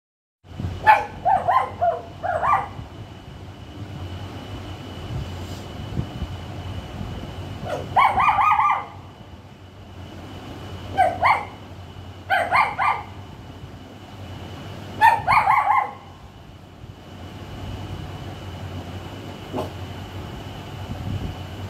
Roxy Pehli baar bark kiya roxy cute smart cute puppy
Roxy Pehli baar bark kiya mast voice